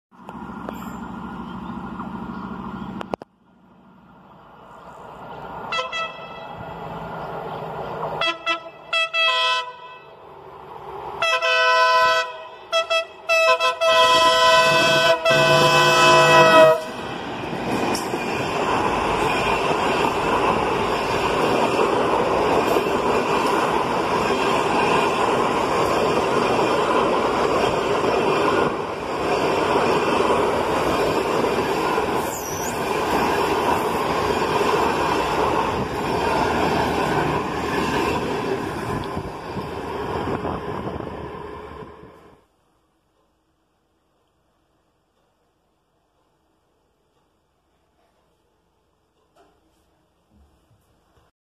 8G 0008 con el 266 a los bocinazos por San Nicolás (0120)27-3-2021
8G 0008 con el 266 a los bocinazos por San Nicolás
8G 0008 with the 266 to the horns for San Nicolás